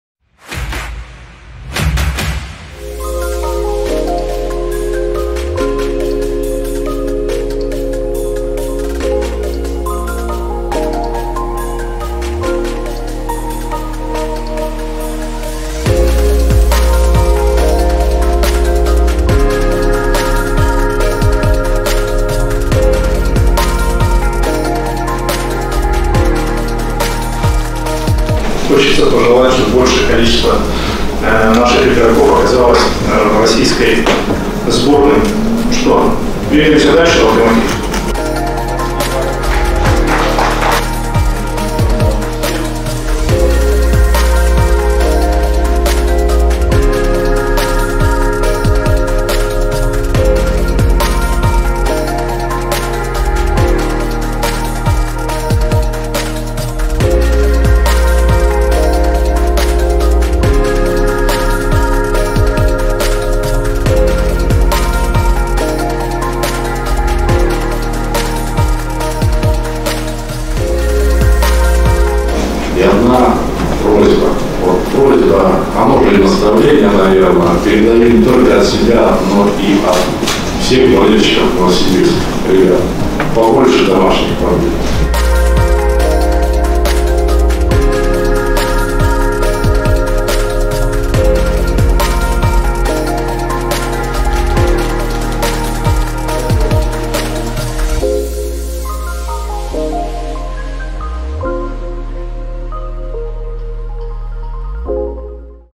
Железнодорожники провели встречу с руководством
Все самое интересное о собрании в Управлении Западно-Сибирской железной дороги прошла в нашем видео – подарки из Токио, фуражки новичкам и совместное фото на память!

Новый сезон. Новые победы. Новая философия.